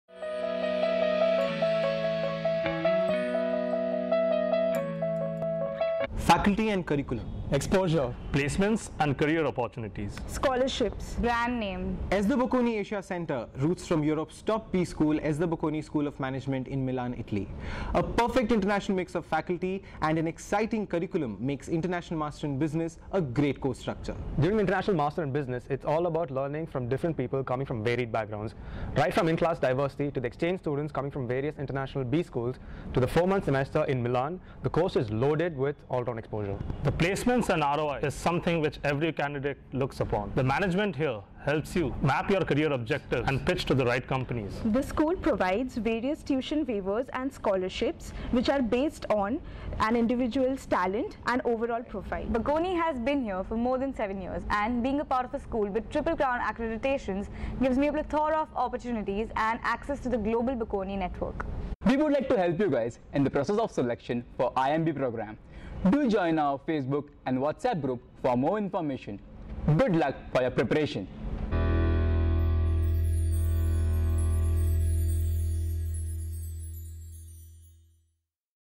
What was your Business School Selection Criteria?
Watch this video to know about the various parameters that are critical for selecting a Business School.